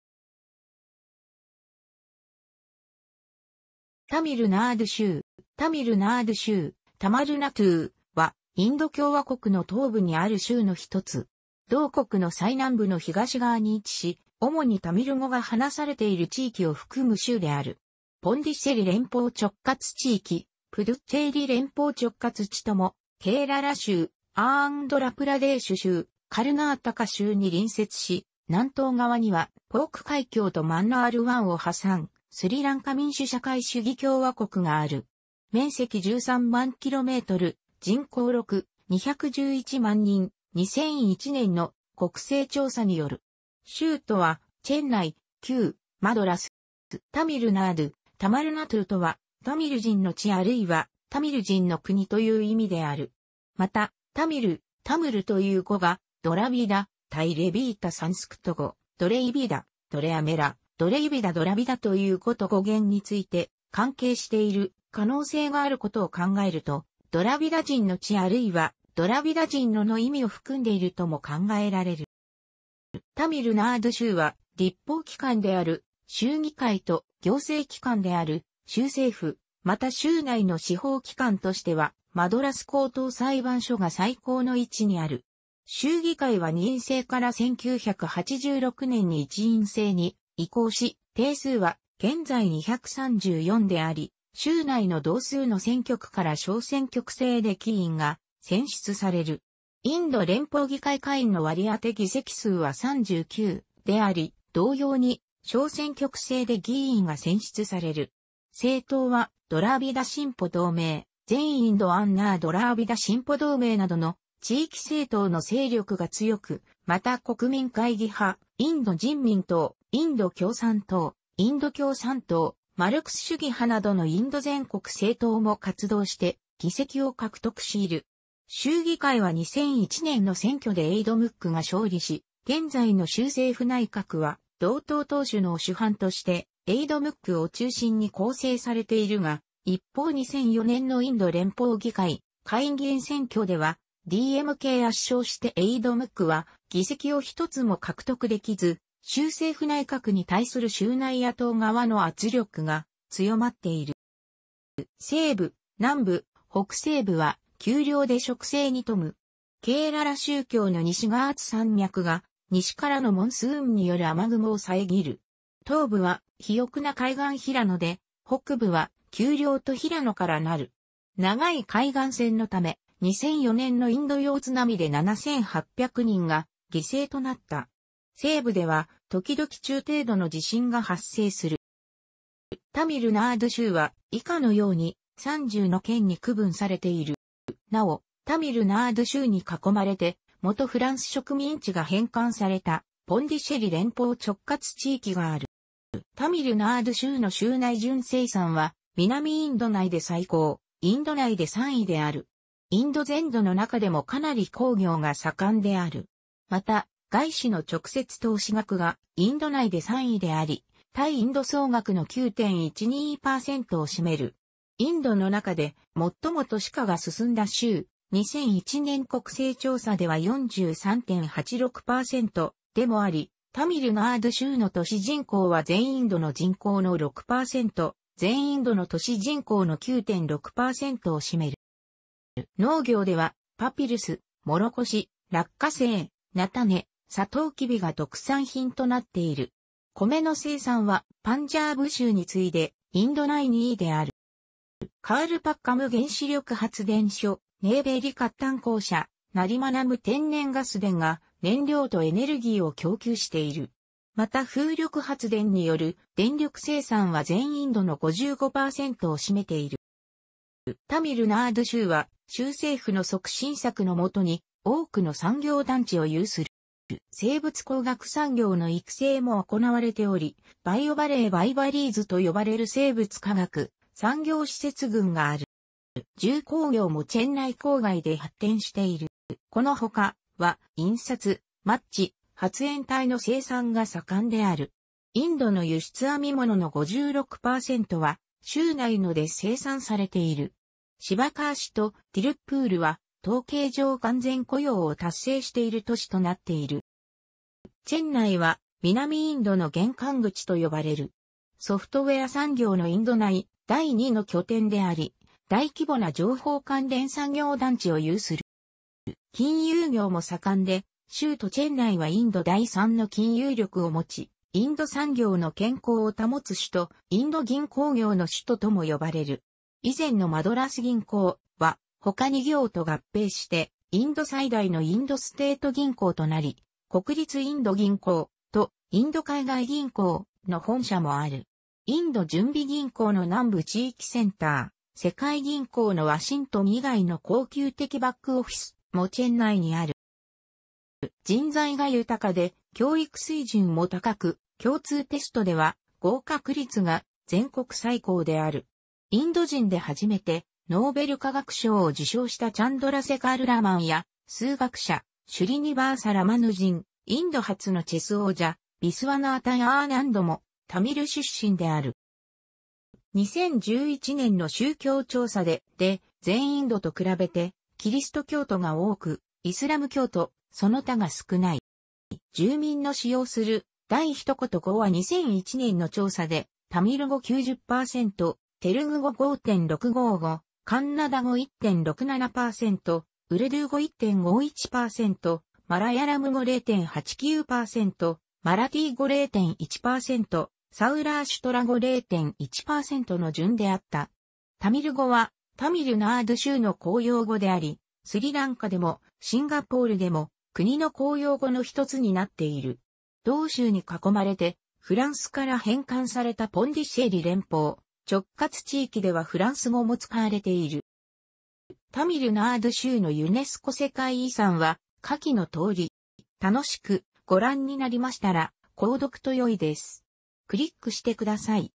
タミル・ナードゥ州
タミル・ナードゥ州（ "tamiḻnāṭu"、）は、インド共和国の東部にある州の一つ。同国の最南部の東側に位置し、主にタミル語が話されている地域を含む州である。ポンディシェリ連邦直轄地域（プドゥッチェーリ連邦直轄地とも）、ケーララ州、アーンドラ・プラデーシュ州、カルナータカ州に隣接し、南東側にはポーク海峡とマンナール湾を挟んでスリランカ民主社会主義共和国がある。面積 13 万km、人口 6,211 万人（2001年の国勢調査による）。州都はチェンナイ（旧マドラス）。

タミル・ナードゥ（ "tamiḻnāṭu"）とは、「タミル人の地」あるいは「タミル人の国」という意味である。また、「タミル」（ "tamiḻ"）という語がドラヴィダ（ "tirāviṭa"；サンスクリット語 द्रविड "draviḍa", द्रमिल "dramila", द्राविड "drāviḍa"；Dravida）という語と語源について関係している可能性があることを考えると、「ドラヴィダ人の地」あるいは「ドラヴィダ人の国」の意味を含んでいるとも考えられる。

タミル・ナードゥ州は、立法機関である州議会と、行政機関である州政府、また州内の司法機関としてはマドラス高等裁判所が最高の位置にある。州議会は二院制から1986年に一院制に移行し、定数は現在234であり、州内の同数の選挙区から小選挙区制で議員が選出される。インド連邦議会下院の割り当て議席数は39であり、同様に小選挙区制で議員が選出される。政党は、ドラーヴィダ進歩同盟 (DMK)、全インド・アンナー・ドラーヴィダ進歩同盟 (AIADMK) などの地域政党の勢力が強く、また国民会議派、インド人民党 (BJP)、インド共産党 (CPI)、インド共産党マルクス主義派 (CPIM) などのインド全国政党も活動して議席を獲得している。州議会は2001年の選挙でAIADMKが勝利し、現在の州政府内閣は同党党首のを首班としてAIADMKを中心に構成されているが、一方2004年のインド連邦議会下院議員選挙ではDMKが圧勝してAIADMKは議席を一つも獲得できず、州政府内閣に対する州内野党側の圧力が強まっている。

西部・南部・北西部は丘陵で植生に富む。ケーララ州境の西ガーツ山脈が西からのモンスーンによる雨雲を遮る。東部は肥沃な海岸平野で、北部は丘陵と平野からなる。長い海岸線のため、2004年のインド洋津波で7800人が犠牲となった。西部では時々中程度の地震が発生する。

タミル・ナードゥ州は、以下のように、30の県に区分されている。

なお、タミル・ナードゥ州に囲まれて、もとフランス植民地が返還された「ポンディシェリ連邦直轄地域」がある。

タミル・ナードゥ州の州内純生産は南インド内で最高、インド内で三位である。インド全土の中でもかなり工業が盛んである。また、外資の直接投資額がインド内で三位であり、対インド総額の9.12%を占める。インドの中で最も都市化が進んだ州（2001年国勢調査では43.86%）でもあり、タミル・ナードゥ州の都市人口は全インドの人口の6%、全インドの都市人口の9.6%を占める。

農業では、パピルス、蜀黍、落花生、菜種、砂糖黍が特産品となっている。米の生産はパンジャーブ州に次いでインド内二位である。

カールパッカム原子力発電所、ネイヴェーリ褐炭公社、ナリマナム天然ガス田が、燃料とエネルギーを供給している。また風力発電による電力生産は全インドの55%を占めている。

タミル・ナードゥ州は、州政府の促進策のもとに多くの産業団地を有する。

生物工学産業の育成も行われており、「バイオバレー Biovalleys」と呼ばれる生物科学産業施設群がある。

重工業もチェンナイ郊外で発展している。

この他、は、印刷、マッチ、発煙体の生産が盛んである。インドの輸出編み物の56%は、州内ので生産されている。シヴァカーシとティルップールは、統計上完全雇用を達成している都市となっている。

チェンナイは「南インドの玄関口」と呼ばれる。ソフトウェア産業のインド内第二の拠点であり、大規模な情報関連産業団地を有する。

金融業も盛んで、州都チェンナイはインド第三の金融力を持ち、「インド産業の健康を保つ首都」・「インド銀行業の首都」とも呼ばれる。以前のマドラス銀行（）は他二行と合併してインド最大のインドステイト銀行となり、国立インド銀行（）とインド海外銀行（）の本社もある。インド準備銀行の南部地域センター、世...